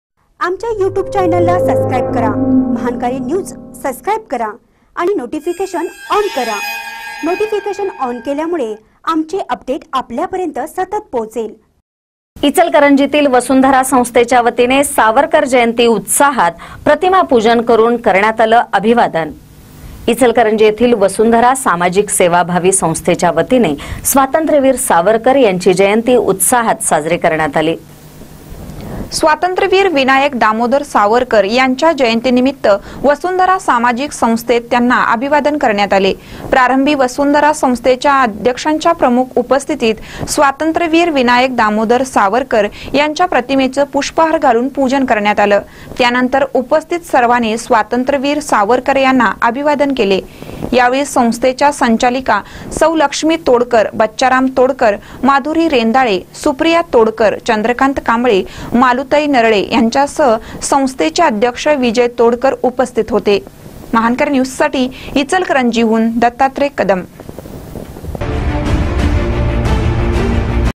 इचलकरंजीतील वसुंधरा संस्थेच्यावतीने सावरकर जयंती उत्साहात
इचलकरंजीतील वसुंधरा संस्थेच्यावतीने सावरकर जयंती उत्साहात : प्रतिमा पूजन करुन करण्यात आले अभिवादन